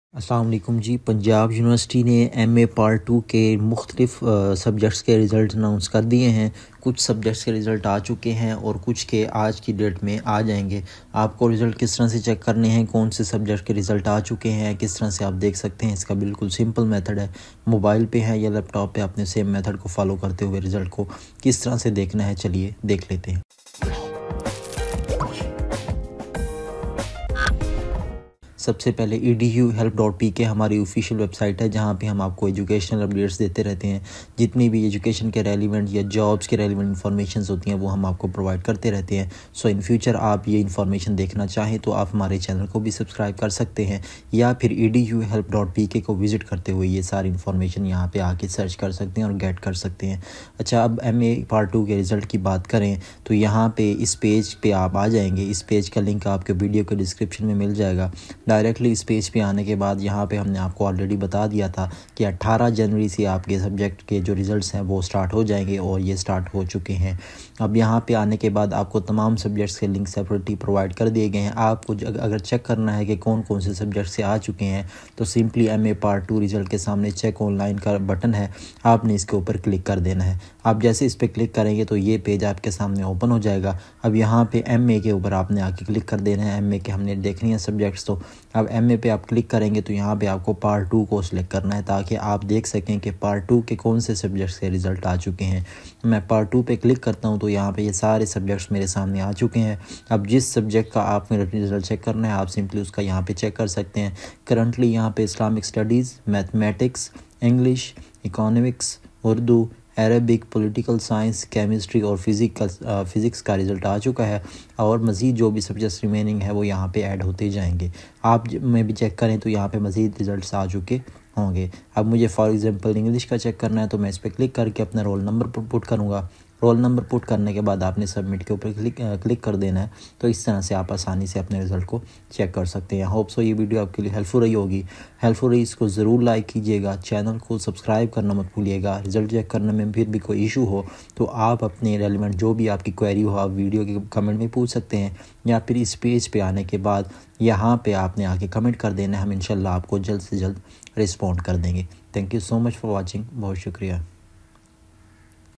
MA Part 2 Result 2024 Punjab University Announced link | PU MA RESULT SUBJECT WISE
Punjab university has announced MA Part 2 Result 2024.